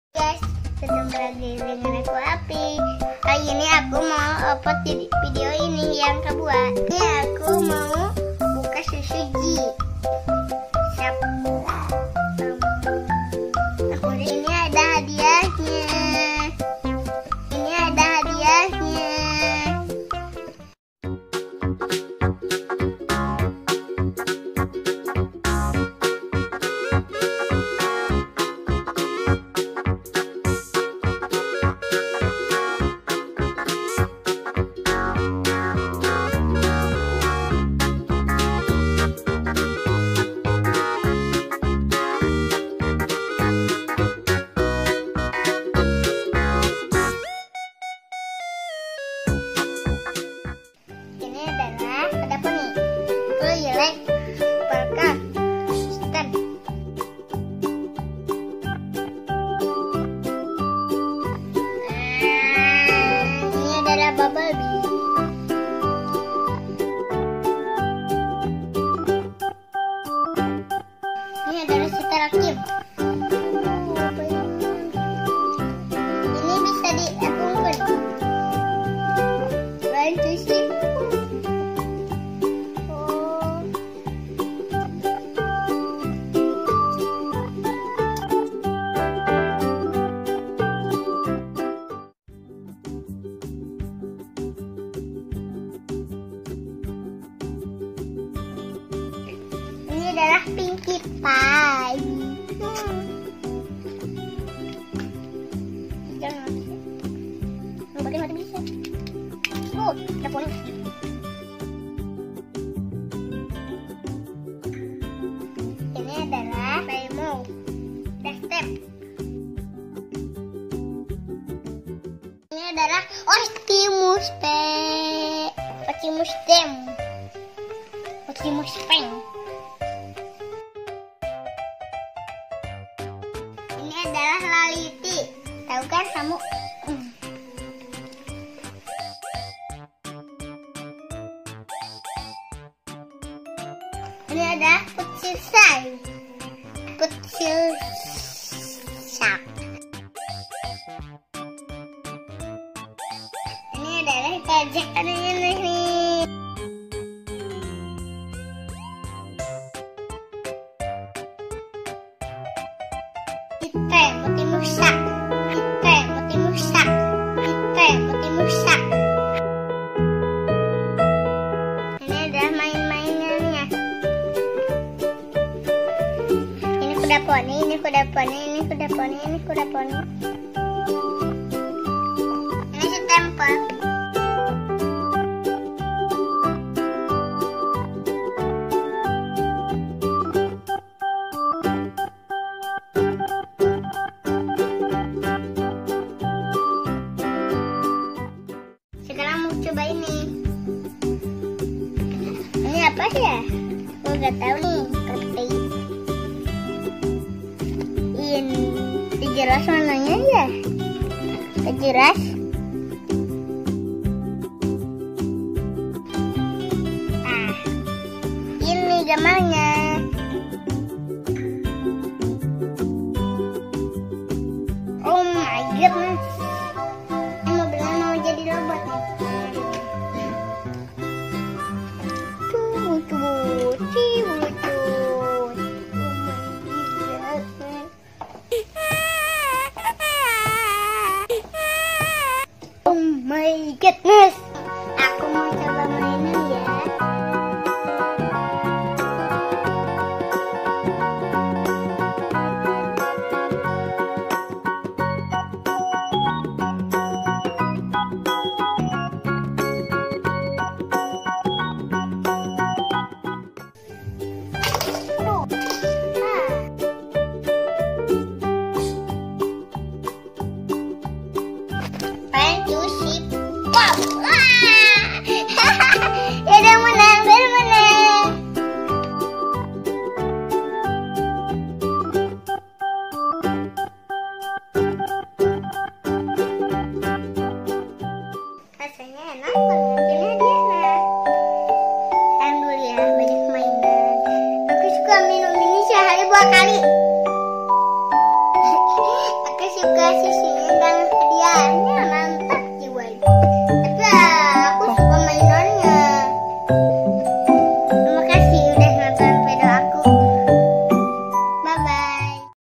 Review Hadiah Susu Zee | Transformer VS Little Pony | Youtuber Cilik
di video ini lebih di khususkan ke review hadiah nya yaitu transformer dan little pony, selain dapat nutrisi dari susu nya kita juga dapat hadiah menarik yang ada didalam kemasa  susu zee dan dengan kelucuan anak 5 tahun dalam berucap.sangat menggemaskan dan semoga terhibur